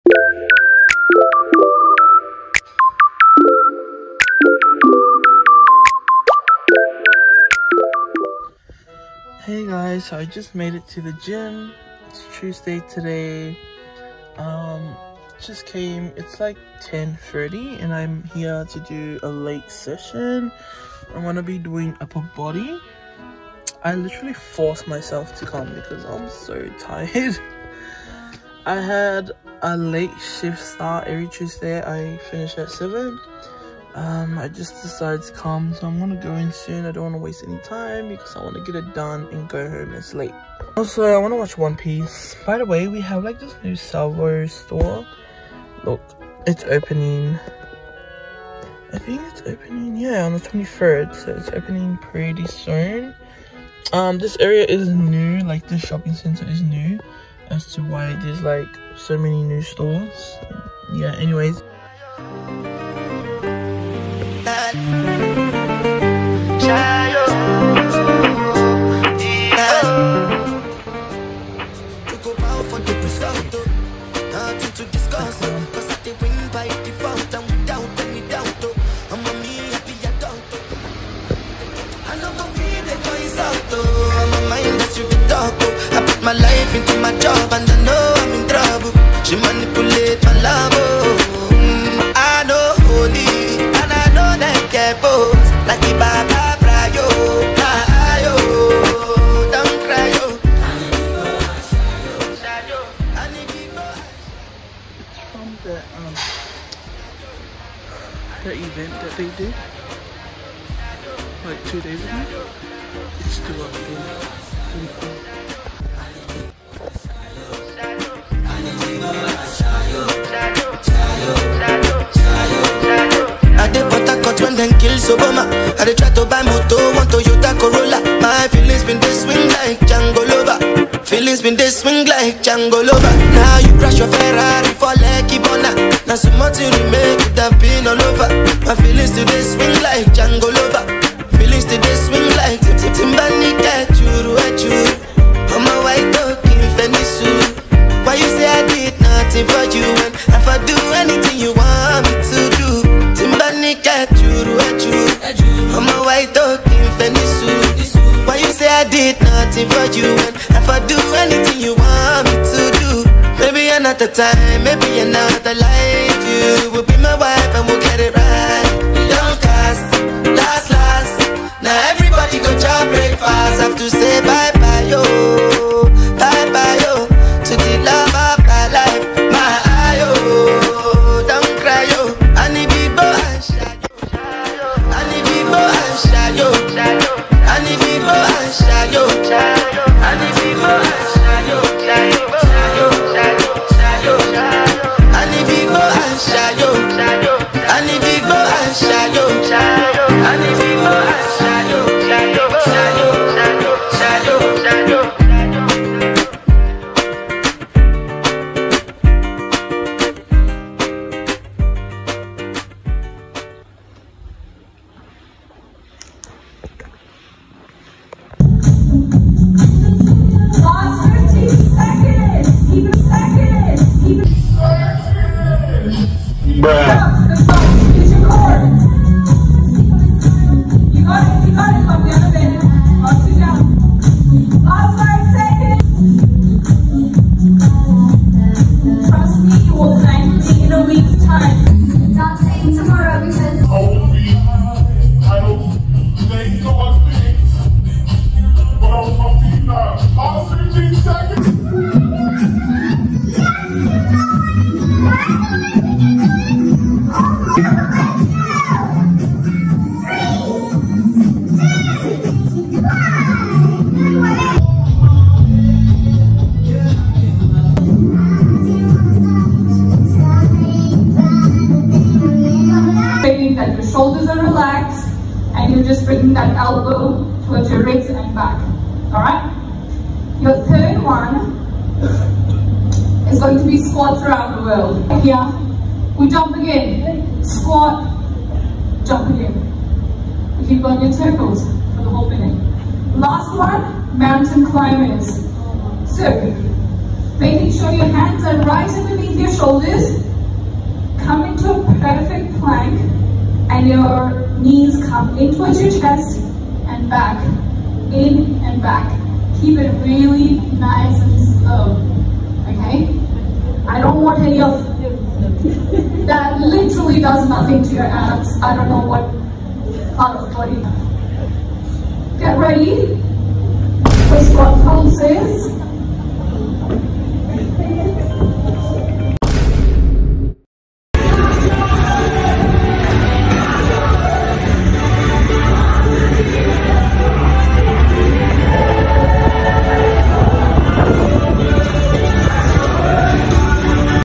Week 5 of Samoan girl tries to lose weight!
12 week weight loss progress still going strong! Stay consistent and disciplined is key & it's easier. The cravings of wanting to eat junk are real but remembering my WHY and why I started this journey keeps me in place.

Lol I was sick that's why I posted so late, but here it is.
My Goal is to hit 100kg by early-mid 2025 lesss goooo

Socials.